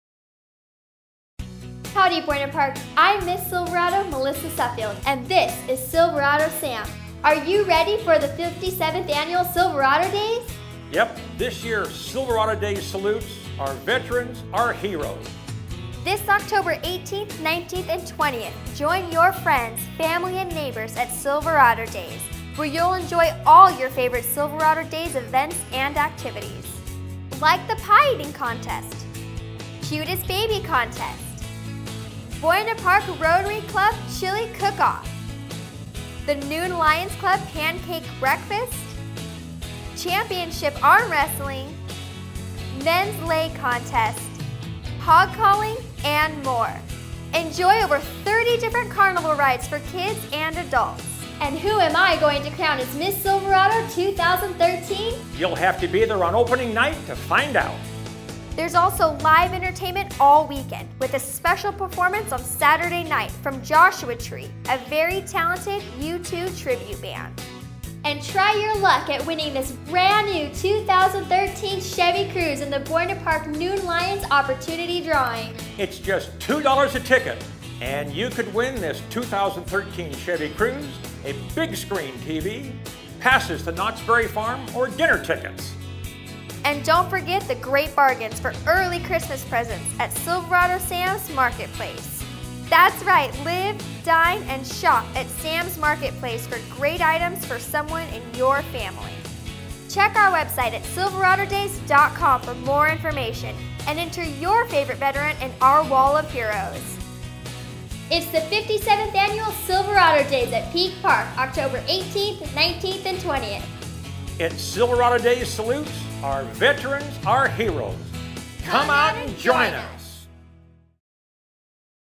Silverado Days 2013 Promo featuring Miss Silverado
It's time for Silverado Days! What are you doing this October? Mark your calendars for October 18, 19, & 20, 2013 and come out to Peak Park for 3 days of fun for the whole family! See you at Silverado Days!